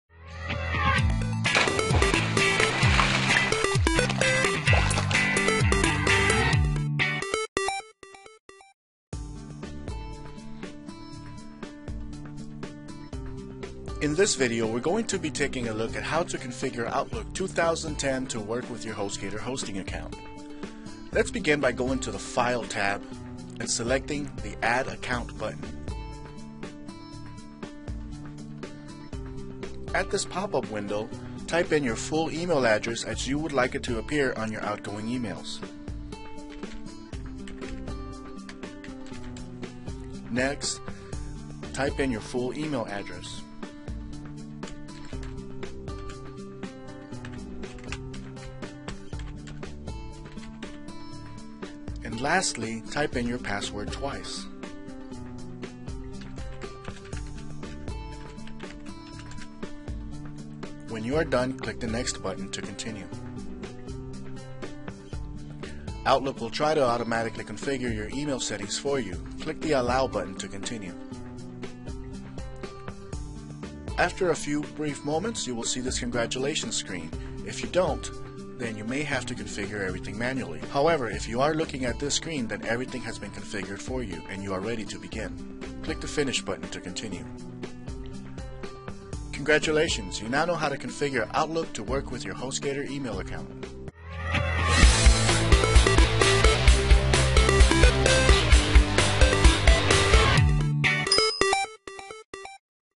Outlook 2010 - Automatic Configuration
This video demonstrates how to automatically configure Outlook 2010 with your Hostgator.com powered email account.